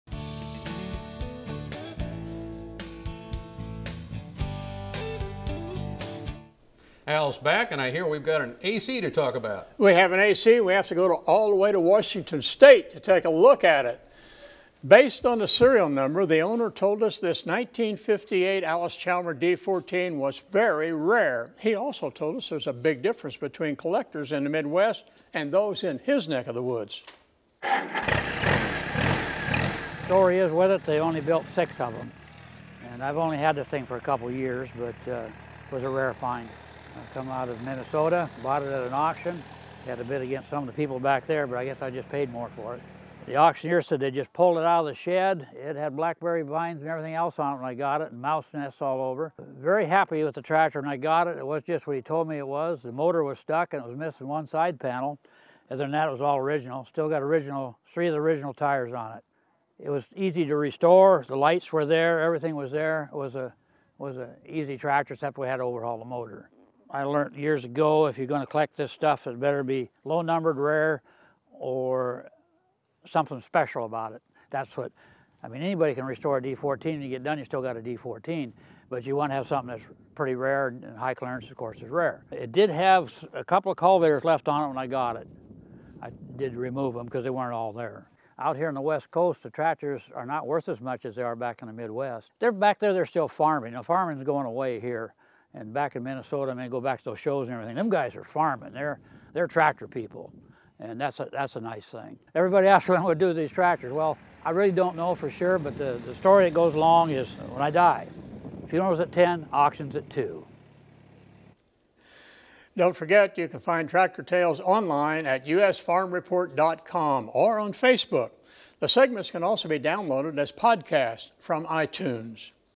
Tractor Tales: 1958 Allis-Chalmers D-14
Based off of it's serial number this 1958 Allis Chalmers D14 is very rare according to it's owner.